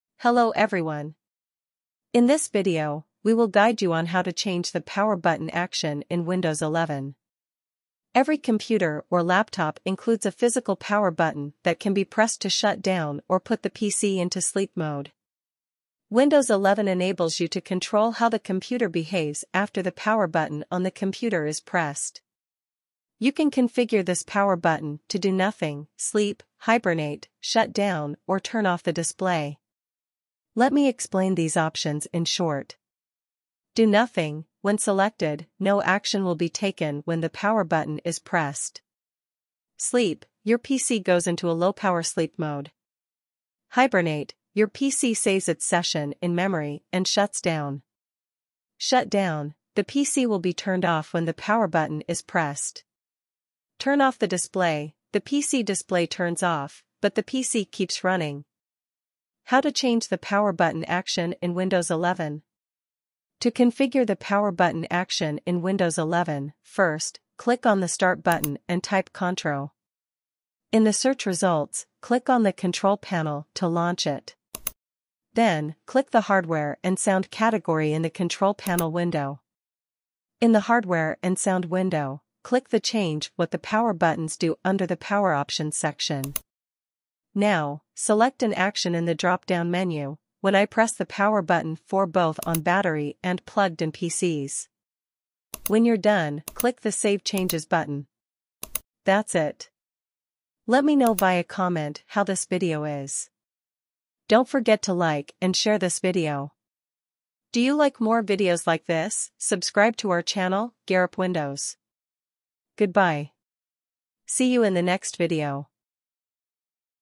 How to Change Power Button Action in Windows 11 | GearUpWindows Tutorial
How to Change Power Button Action in Windows 11?

Every computer or laptop includes a physical power button that can be pressed to shut down or put the PC into sleep mode. Windows 11 enables you to control how the computer behaves after the Power button on the computer is pressed. You can configure this Power button to Do nothing, Sleep, Hibernate, Shut down or Turn off the Display.

Do Nothing: When selected, no action will be taken when the Power button is pressed.
Sleep: Your PC goes into a low-power sleep mode.
Hibernate: Your PC saves its session in memory and shuts down.
Shut Down: The PC will be turned off when the Power button is pressed.
Turn off the Display: The PC display turns off, but the PC keeps running.

How to Change the Power Button Action in Windows 11?

To configure the Power button action in Windows 11, first, click on the Start button and type Contro.

In the search results, click on the Control Panel to launch it.

Then, click the Hardware and Sound category in the Control Panel window.

In the Hardware and Sound window, click the Change what the power buttons do under the Power Options section.

Now, select an action in the drop-down menu, “When I press the power button” for both “On battery” and “Plugged in” PCs.

When you’re done, click the Save changes button.

That's it.

Let me know via comment how this video is.

Chapters

00:00 Intro
00:58 How to Change the Power Button Action in Windows 11?